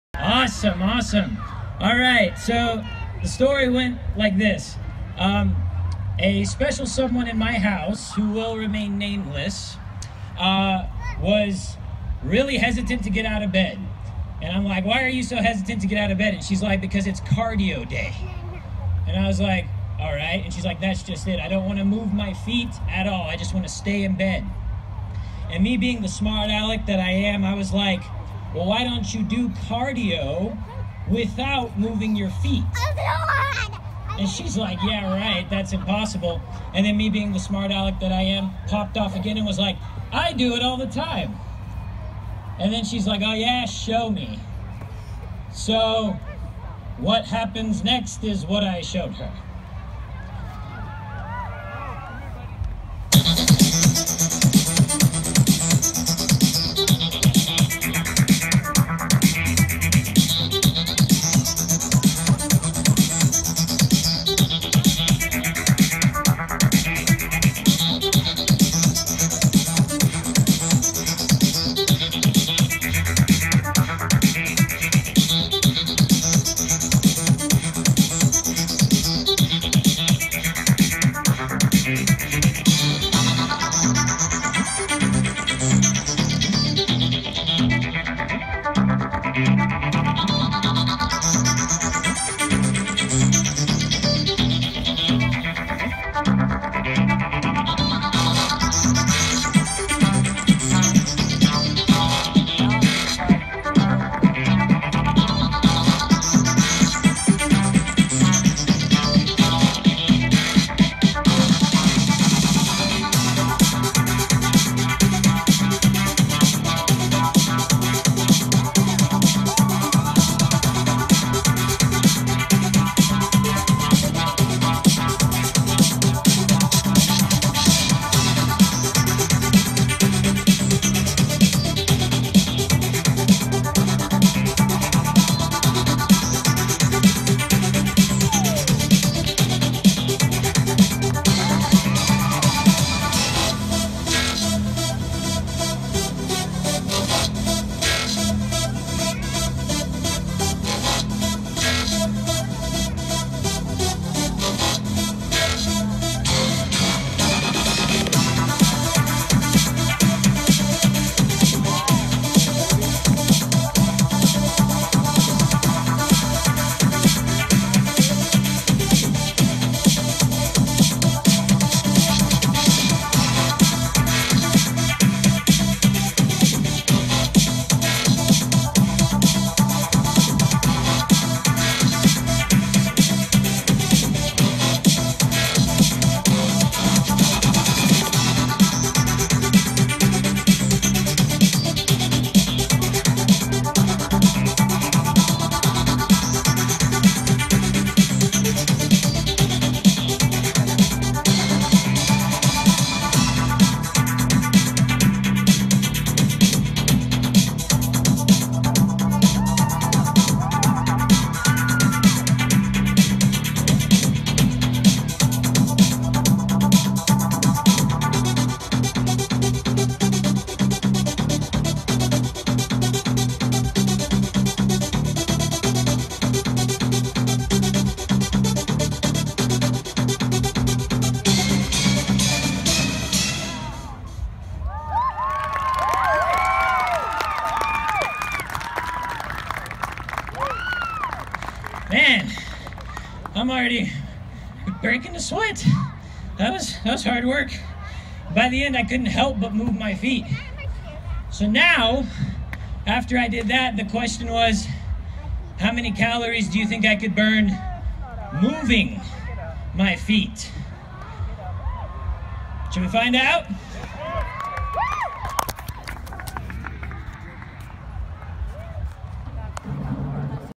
Matthew DelaFuente LIVE: Dance/Performance at Carnival
This was my live performance/dance at the steel days carnival in American Fork.